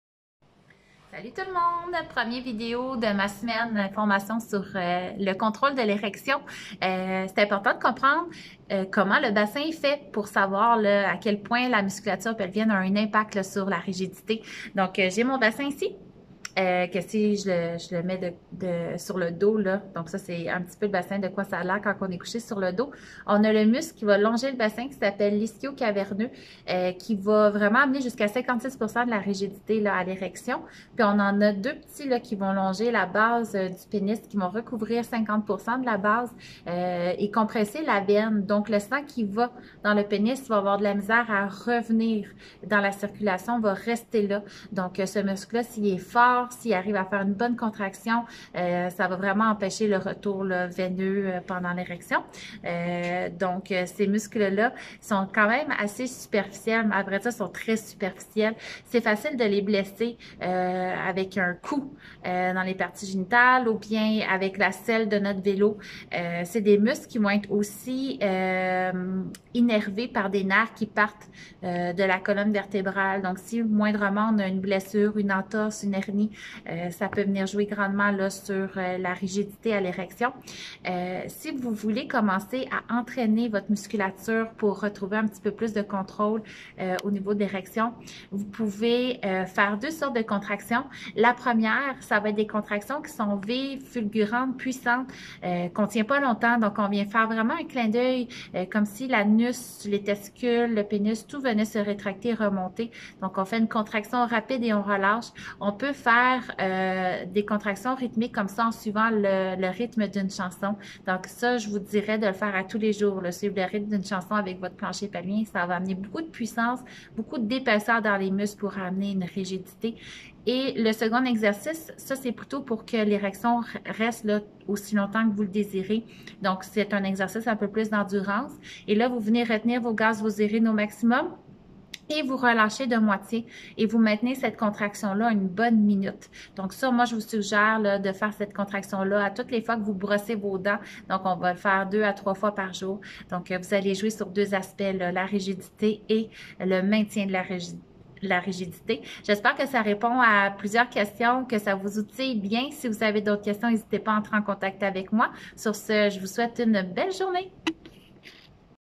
Le contrôle érectile, c'est musculaire!
Presque 60% de la rigidité provient des muscles!

Je te donne deux exercices en prime.

Brise les tabous.

Reprends le contrôle.